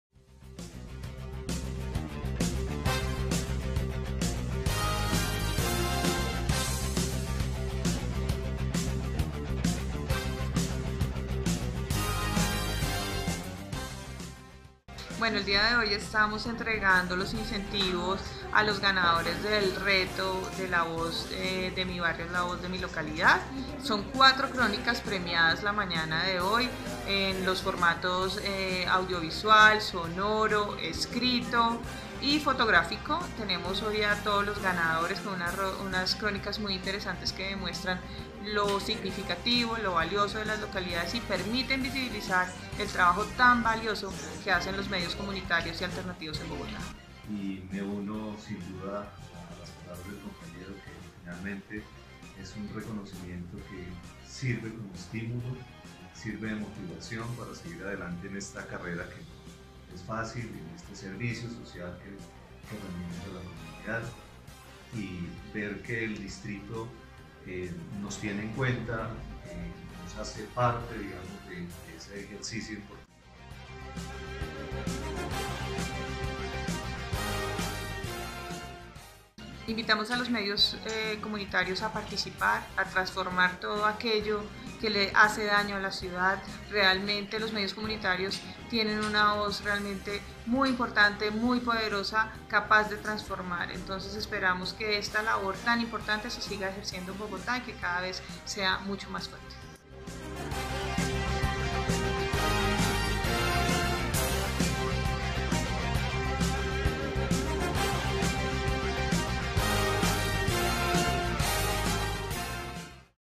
Mejor Crónica Escrita para El Corazón de tu Ciudad
El Corazón de tu Ciudad resultó ganador en concurso organizado por el IDPAC denominado Reto Mi Localidad, la Voz de mi Ciudad, se premiaron cuatro categorías, Audiovisual, Sonoro, Fotográfico y Escrito.